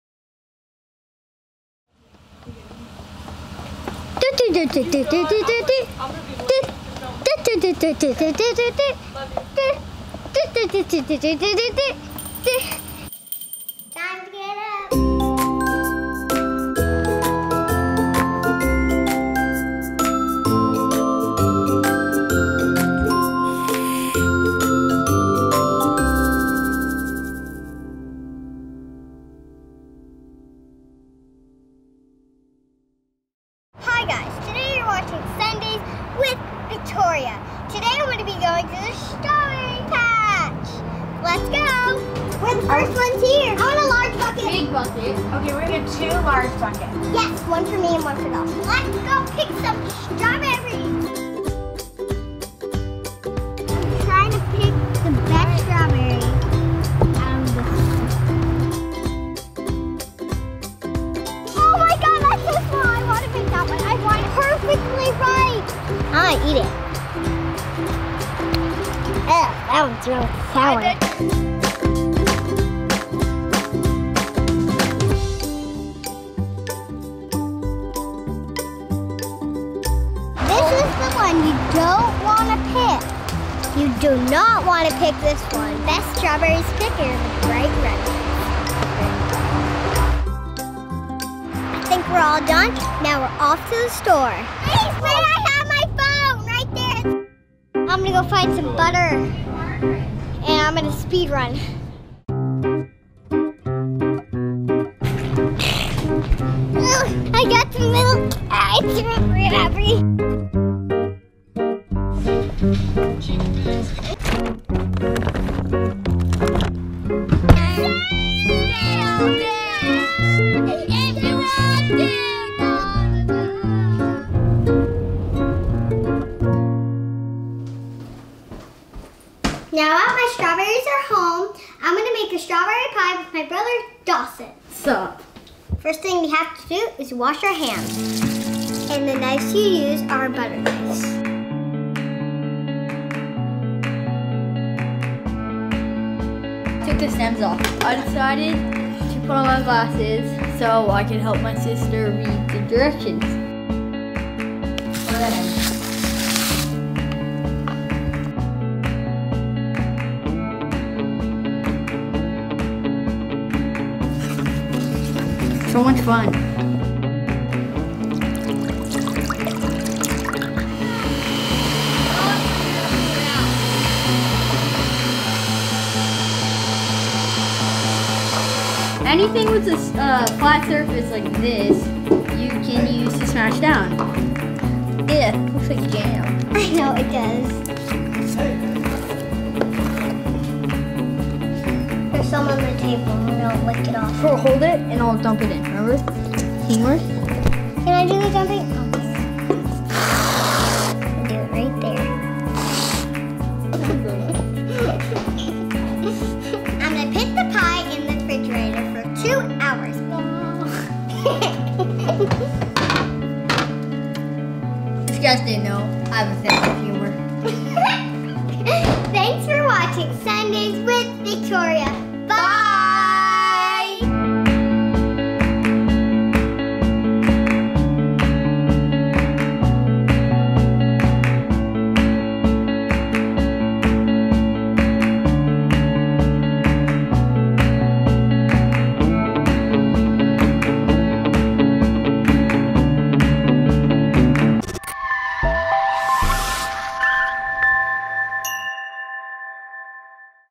Sundays with Victoria: Strawberry Pie
Hi guys! It's Sundays with Victoria, and today I'm making strawberry pie with my brother Dawson. It's so much fun picking the berries! Use our recipe to make your own strawberry pie - Yum!